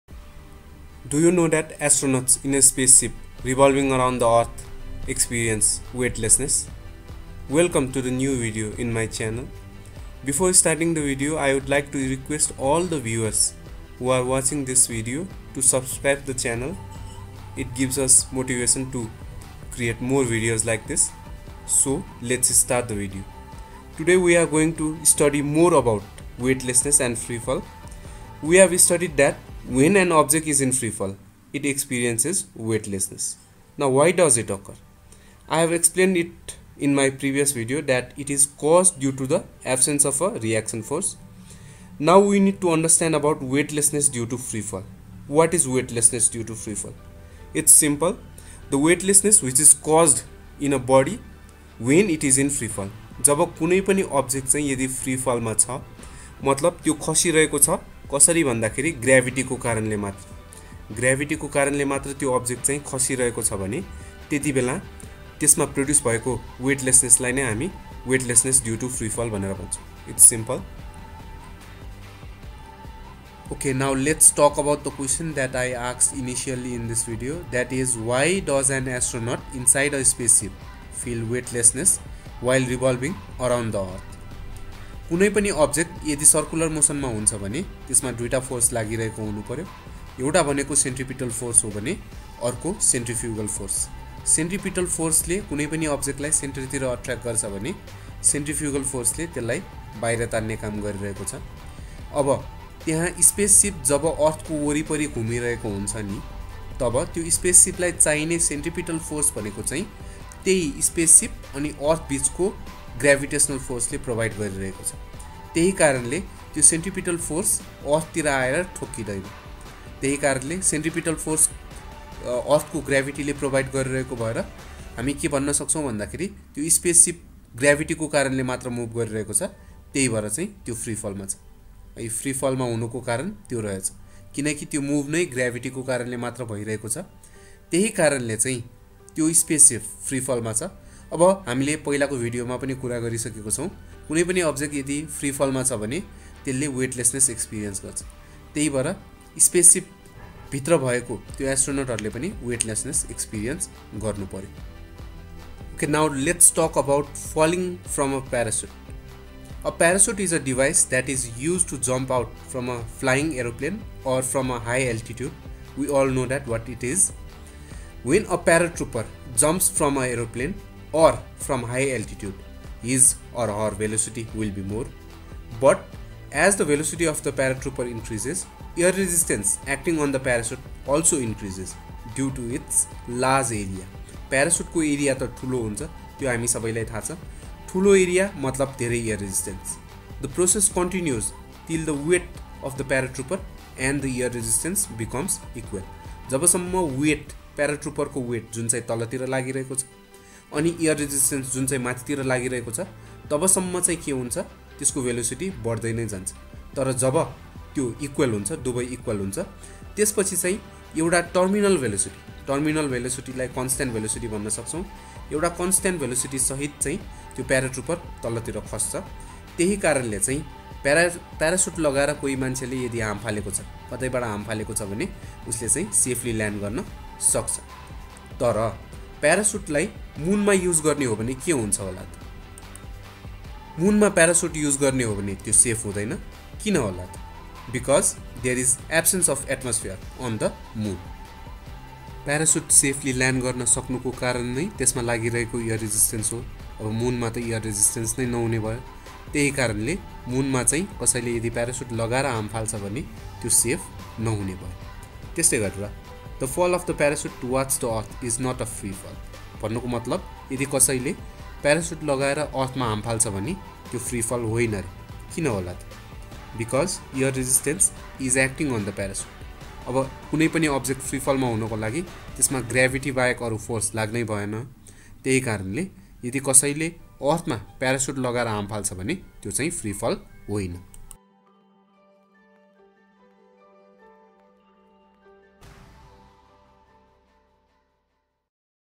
Why does an astronaut inside an spaceship feel weightlessness? More about Weightlessness
In this video I have explained about the cause of weightlessness that a person experiences while being in a spaceship revolving around the earth. This question has been asked in many of the exams so I hope by watching the video, you will able to gather more information on the topic. Also the fall of parachute on the earth and on the moon has been explained.